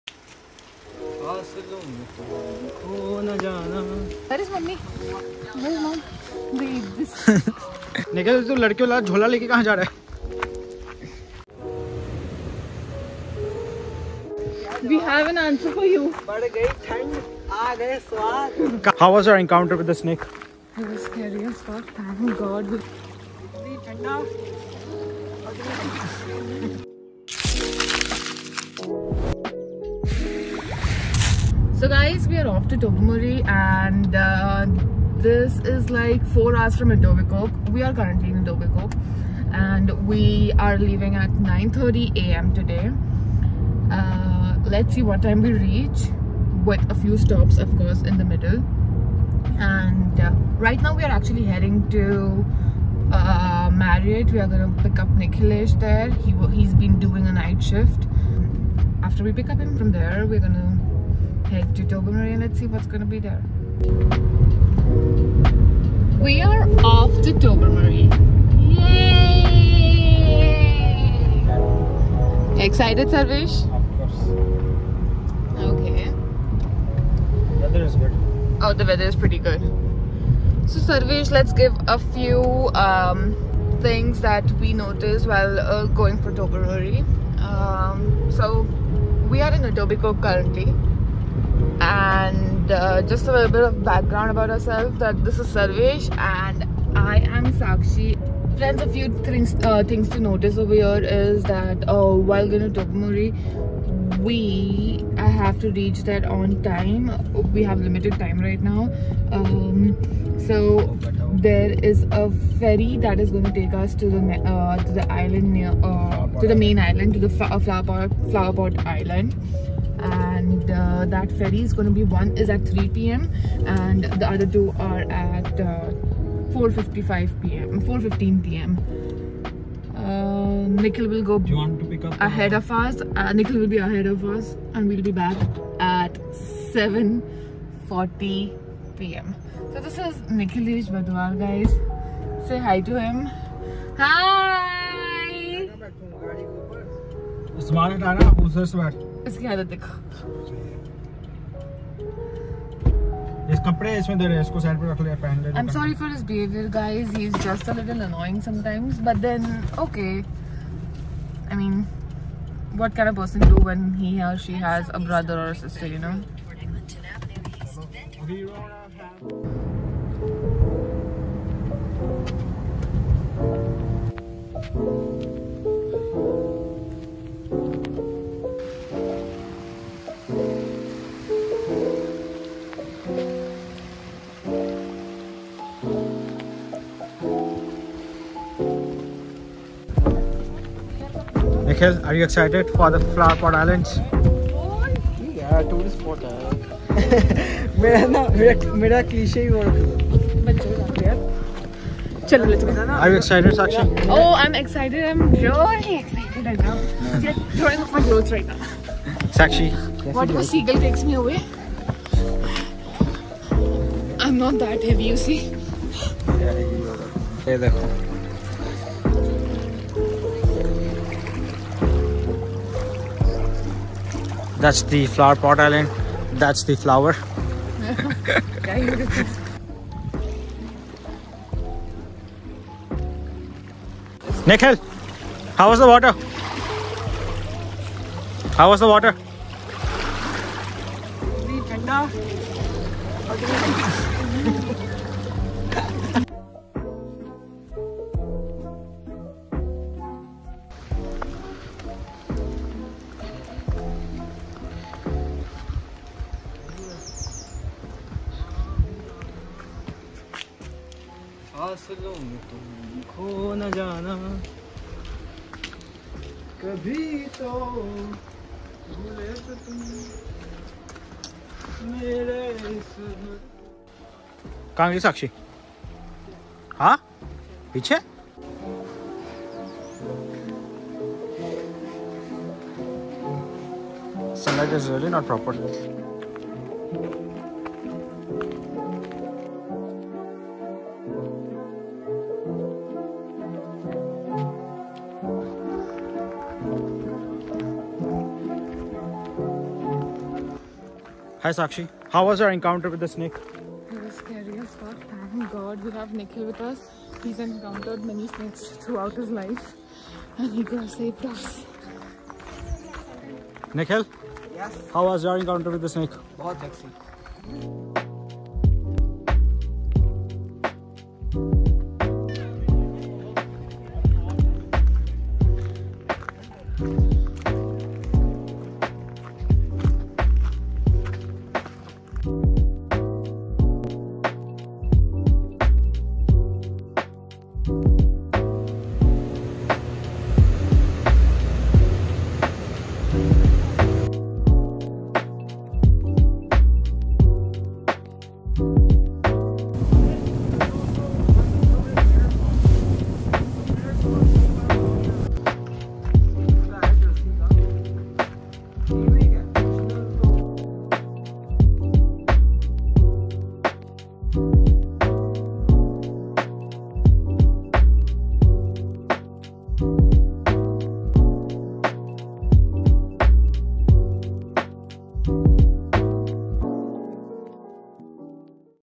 Tobermory and Flower Pot Island | Canada Travel Vlog
Join us in our weekend trip to Tobermory. The flower pot island offers stunning views along with crystal blue waters. In order to reach the island, one needs to book a ferry which departs every few minutes from the town.